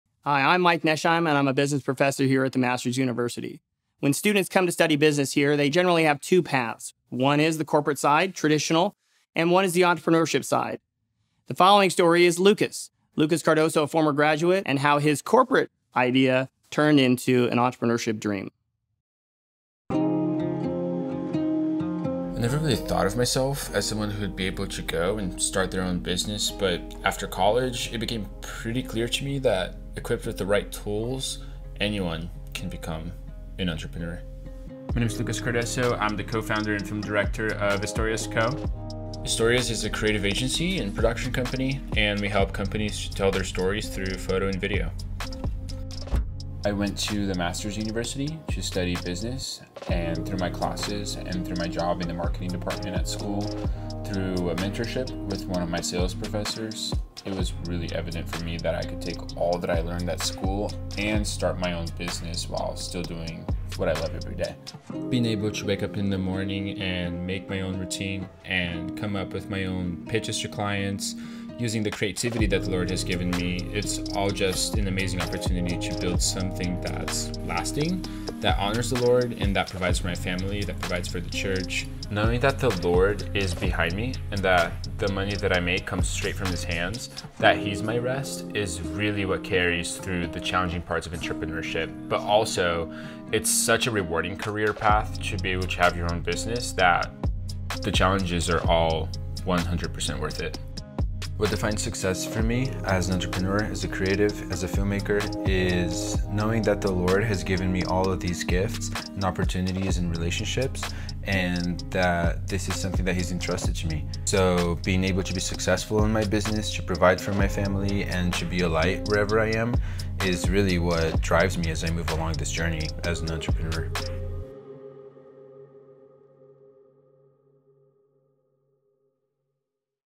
Entrepreneurship at The Master's University
Are you called to a career involving the starting and managing of a business? The Master’s University offers four specialized entrepreneurship tracks: small business, non-profit, media production, and tech.